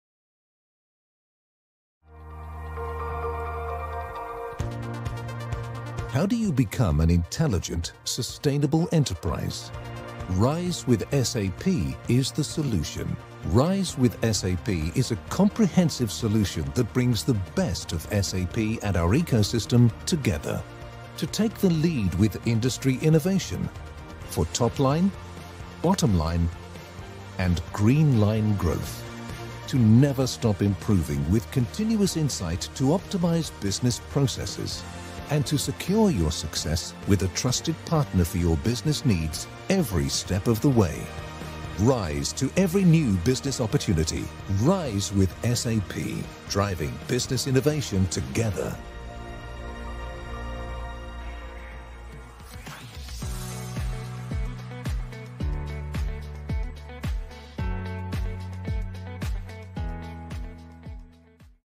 Partner with ExD - RISE with SAP
Partner with ExD to harness RISE with SAP and become an intelligent, sustainable enterprise. Drive transformation and unlock new opportunities for growth and efficiency.